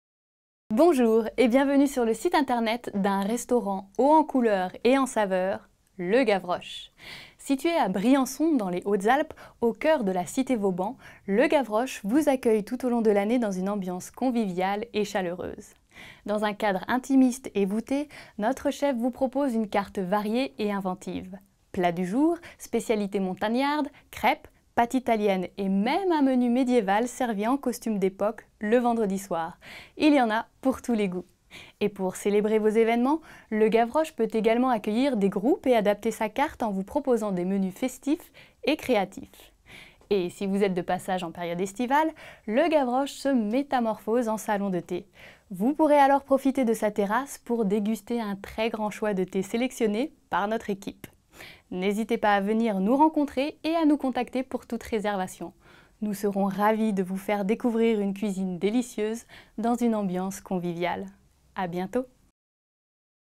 Spécialité montagnarde, cuisine traditionnelle - RESTAURANT LE GAVROCHE
Situé à Briançon dans les Hautes-Alpes, au coeur de la Cité Vauban, Le Gavroche vous accueille tout au long de l'année dans une ambiance conviviale et chaleureuse.

Dans un cadre intimiste et voûté, notre chef vous propose une carte variée et inventive. Plats du jour, spécialités montagnardes, crêpes, pâtes italiennes et même un menu médiéval servi en costume d'époque le vendredi soir. Il y en a pour tous les goûts!!

Et pour célébrer vos évènements, Le Gavroche peut également accueillir des groupes et adapter sa carte en vous proposant des menus festifs et créatifs.

Et si vous êtes de passage en période estivale, le Gavroche se métamorphose en salon de thé. Vous pourrez alors profiter de sa terrasse pour déguster un très grand choix de thés sélectionnés par notre équipe.

N'hésitez pas à venir nous rencontrer et à nous contacter pour toute réservation. Nous serons ravis de vous faire découvrir une cuisine délicieuse dans une ambiance conviviale.